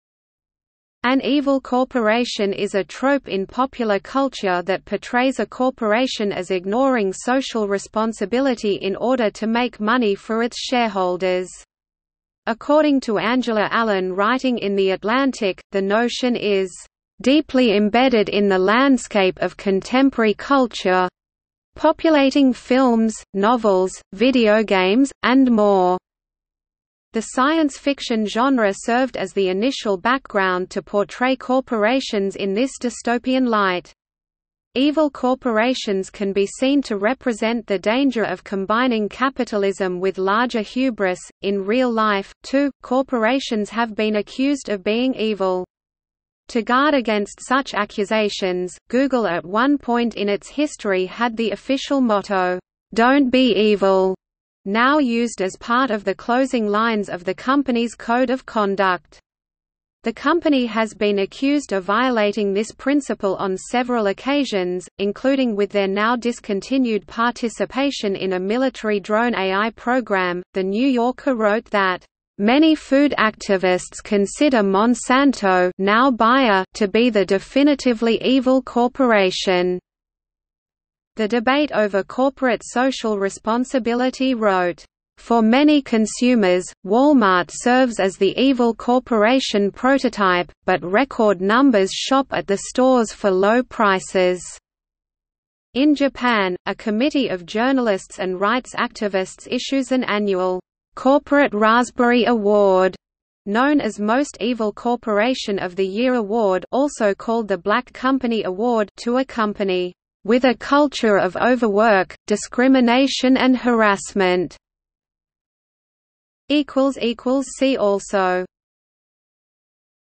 Evil corporation | Wikipedia audio article
- Socrates

SUMMARY
An evil corporation is a trope in popular culture that portrays a corporation as ignoring social responsibility in order to make money for its shareholders. According to Angela Allan writing in The Atlantic, the notion is "deeply embedded in the landscape of contemporary culture—populating films, novels, videogames, and more." The science fiction genre served as the initial background to portray corporations in this dystopian light. Evil corporations can be seen to represent the danger of combining capitalism with larger hubris.In real life, too, corporations have been accused of being evil. To guard against such accusations, Google at one point in its history had the official motto "Don't be evil", now used as part of the closing lines of the company's code of conduct. The company has been accused of violating this principle on several occasions, including with their now discontinued participation in a military drone AI program.The New Yorker wrote that "many food activists consider Monsanto (now Bayer) to be the definitively evil corporation". The Debate over Corporate Social Responsibility wrote, "For many consumers, Wal-Mart serves as the evil corporation prototype, but record numbers shop at the stores for low prices." In Japan, a committee of journalists and rights activists issues an annual "corporate raspberry award" known as Most Evil Corporation of the Year Award (also called the Black Company Award) to a company "with a culture of overwork, discrimination and harassment".